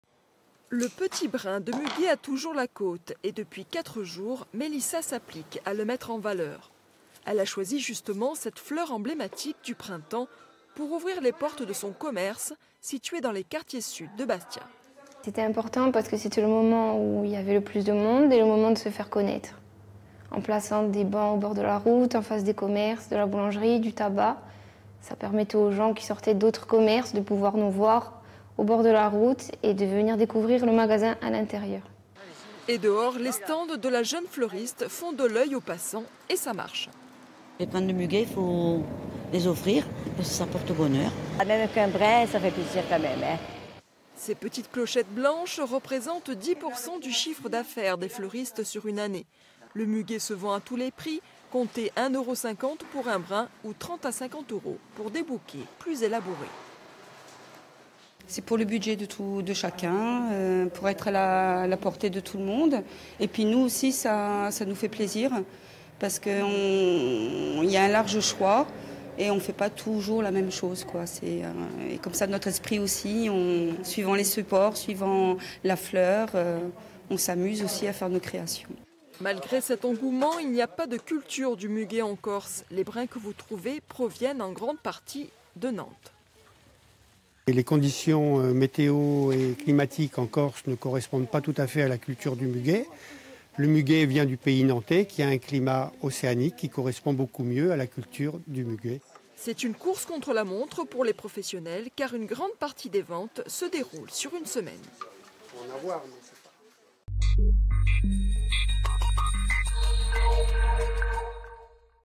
1er mai : chez les fleuristes, les brins de muguet sont prêts à être vendus
Ce mercredi 1er mai, les fleuristes sont prêts à vendre leurs brins de muguet. Les brins seront proposés en boutique ou sur les bords de route. Ces ventes peuvent représenter 10 % du chiffre d'affaires annuel. Illustration à Bastia.

Intervenants - Mélissa Oualit, Fleuriste ; Sabine Vanpepersbraete, Fleuriste.
Equipe - Solange Graziani ; Marion Fiamma ; Ramsey Kinany.